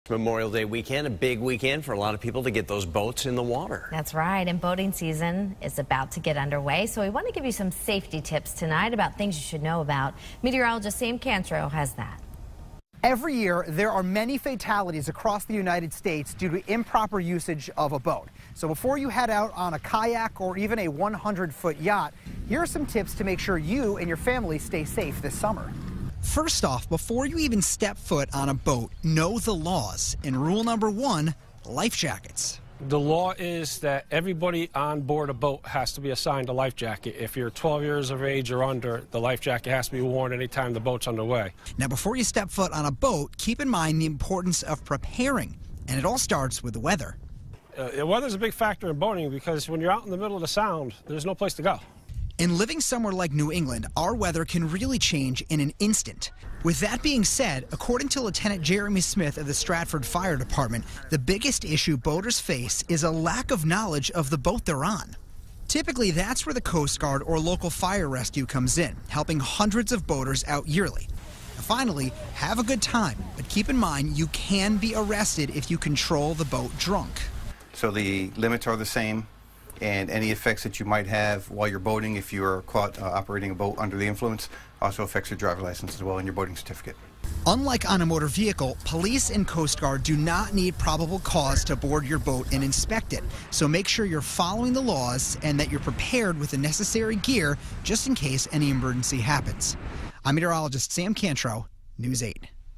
Boating safety tips
Every year there are many fatalities across the US due to improper usage of a boat.